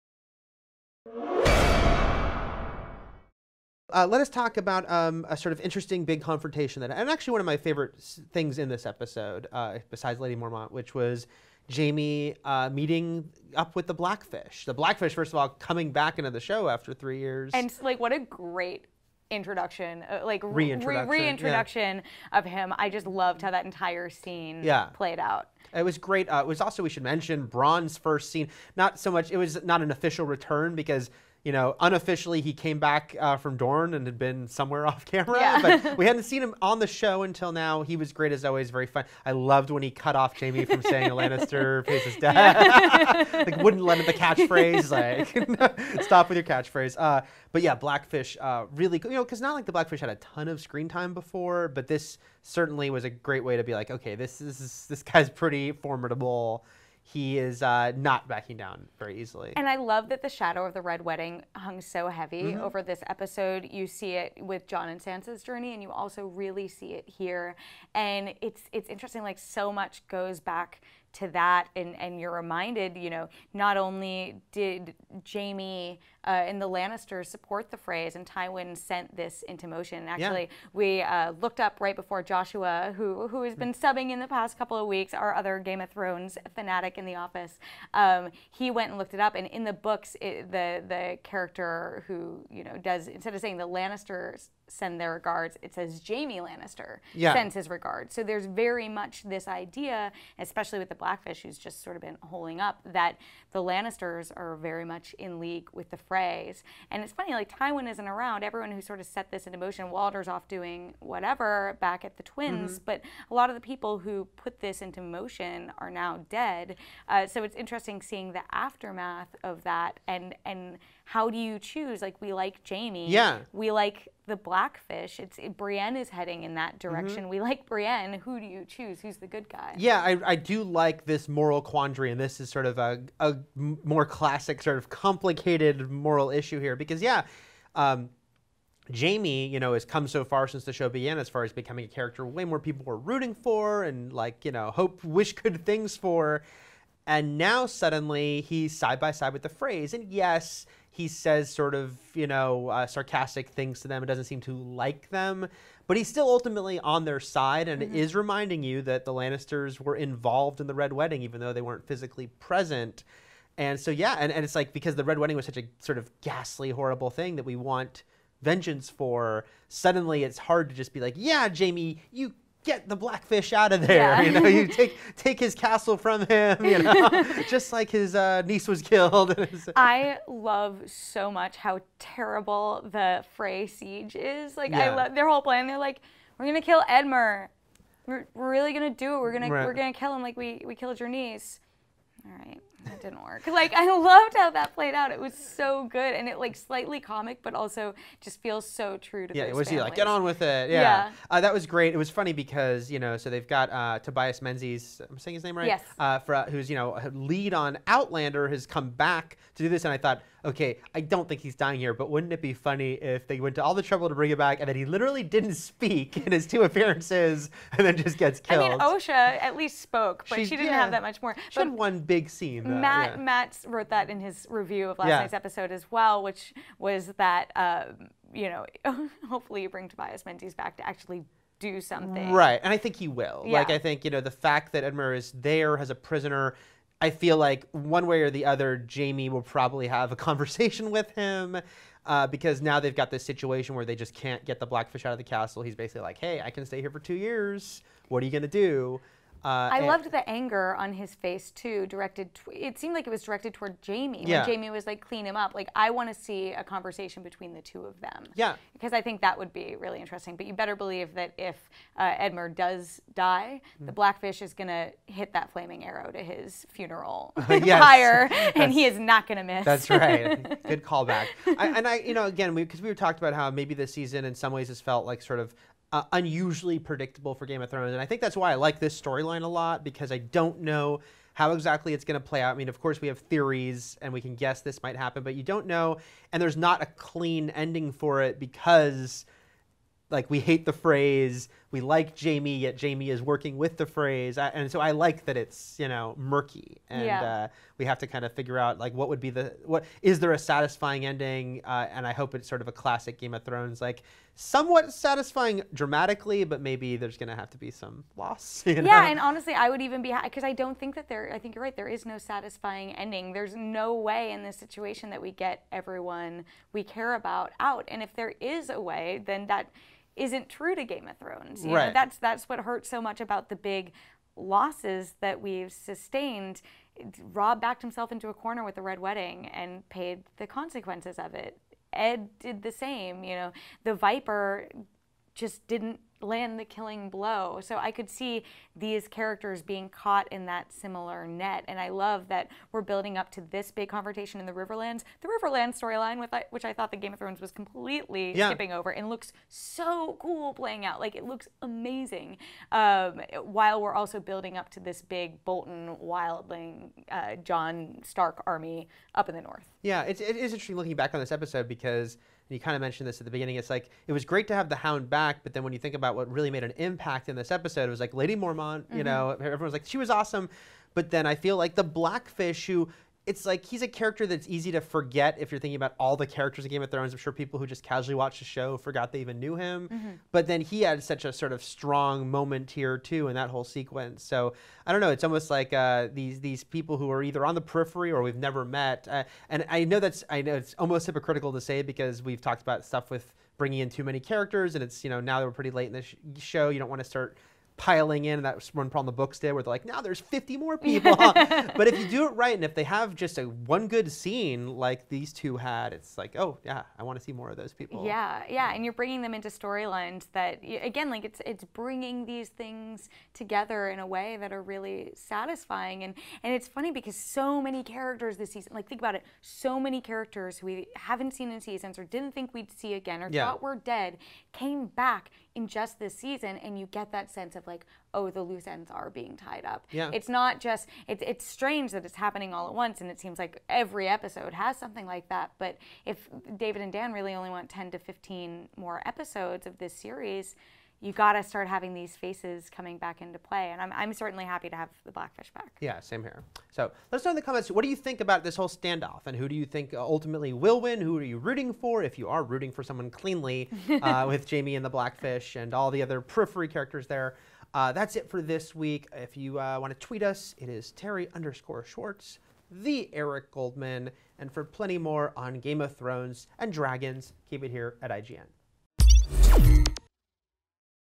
Game of Thrones - No One Wins in the Riverrun Faceoff - Dragons on the Wall
Spoilers! Terri and Eric can't decide who they want to win control of the Riverlands. Is there any happy ending here?

Follow IGN for more!